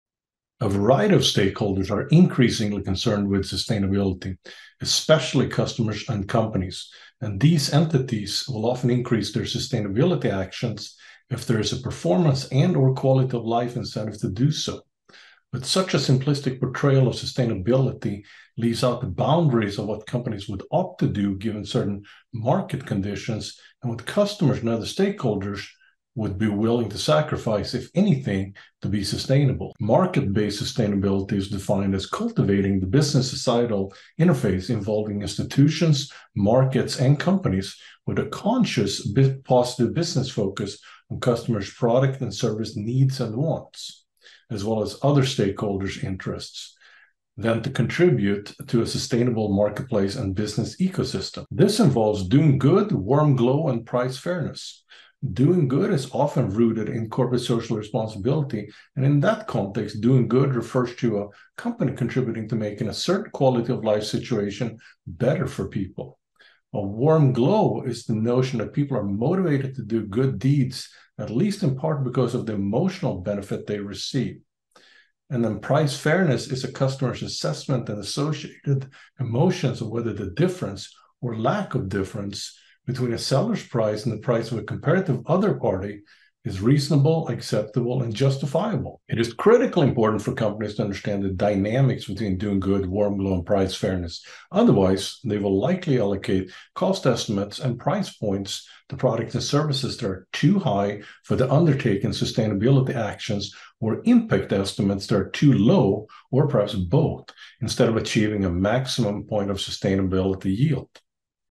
Market-Driven Sustainability
Firms and customers often prioritize sustainability when positive incentives from doing so are clear. But this simplified view neglects market-driven limits on firms and customer trade-offs. The business solution lies in balancing doing good, warm glow, and price fairness.